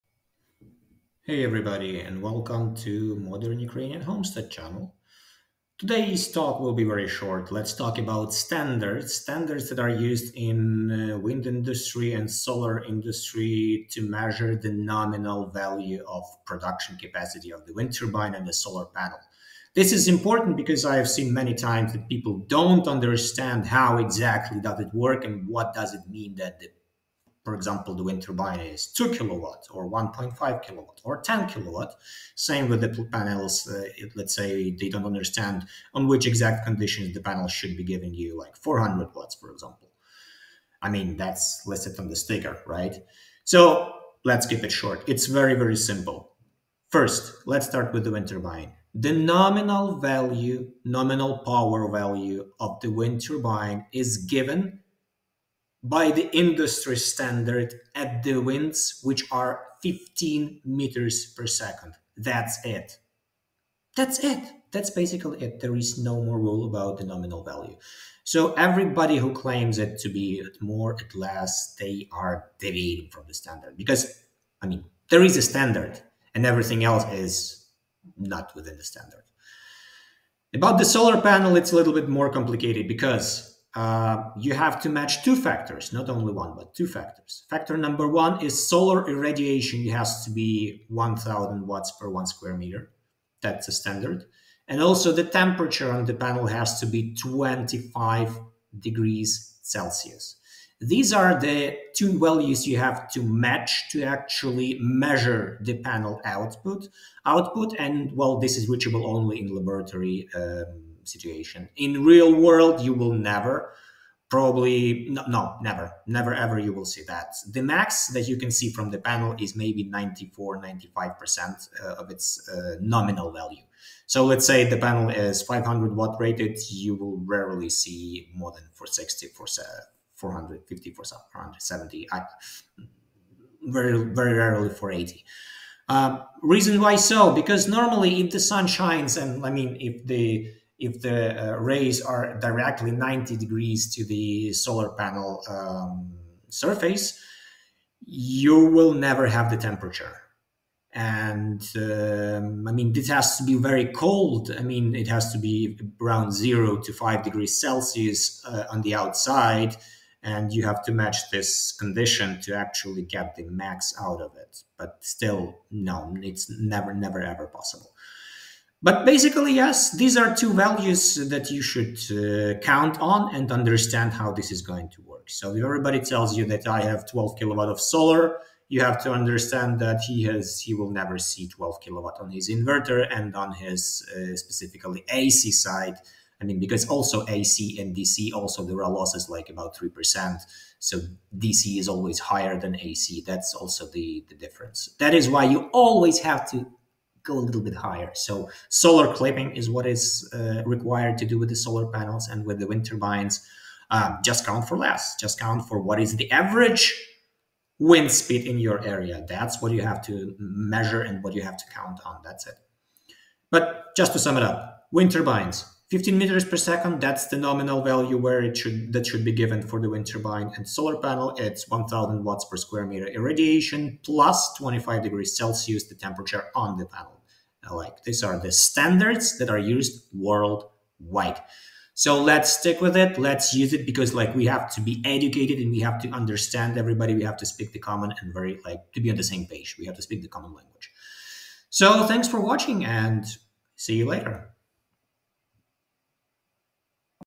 What does "nominal power" of the wind turbine and solar panel mean - checking the standards.
To sum it up shortly on the standards that are used in the industry:
1. For the wind turbines - the nominal power output value of the wind turbine should be given at 15 m/s wind speed.
2. For the solar panel - the nominal power value of the solar module or unit should be given at both parameters combined: solar irradiation should be 1000 W/m2 and the temperature on the panel should be 25 degrees Celsius.

So, whatever I am saying in my videos about power - I stick with these standards and use the nominal values.